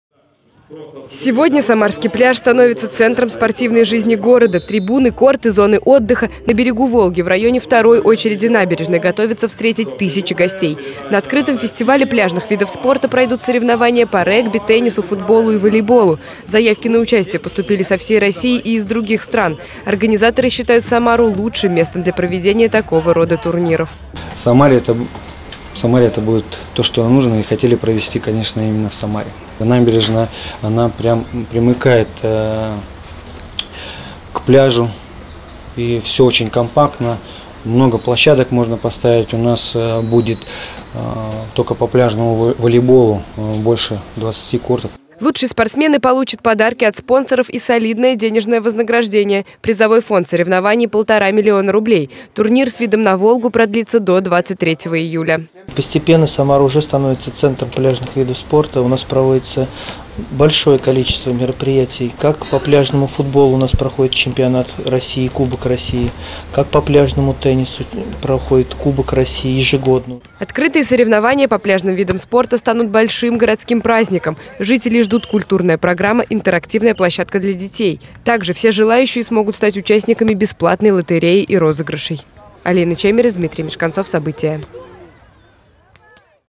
Физкультура на берегу Волги. В Самаре стартуют открытые соревнования по пляжным видам спорта
Волейбол, футбол, теннис и регби - стать участниками турнира могут все желающие. Уже поступило более тысячи заявок. В том числе из других регионов и стран ближнего зарубежья. Игры уже начались, но официальное открытие соревнования пройдет в субботу. Что ждет спортсменов и болельщиков.